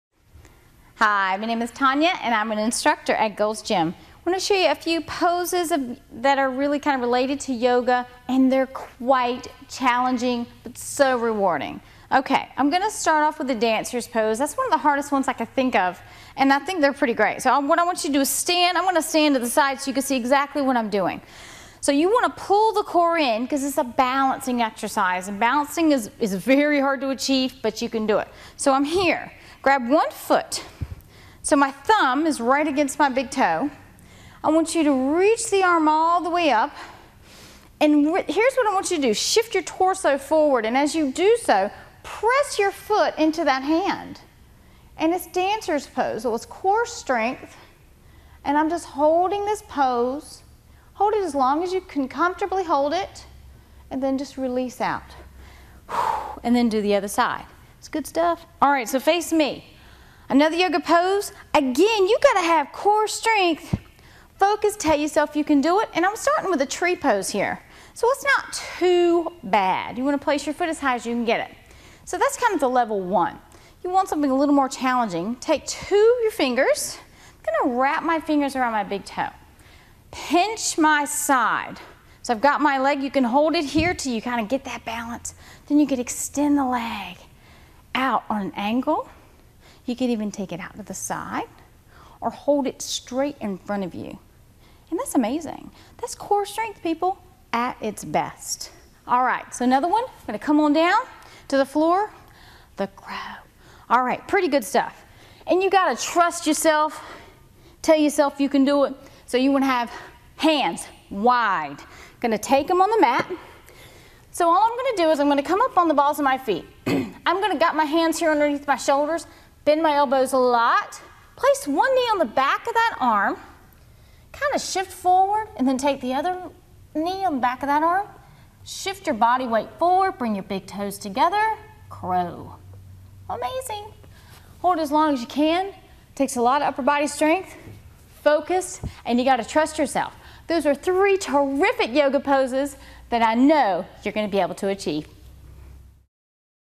The Hardest Yoga Poses
The Hardest Yoga Poses. Part of the series: Keep in Shape. The hardest yoga poses can also be the most rewarding. Find out about the hardest yoga poses with help from a certified personal fitness trainer in this free video clip.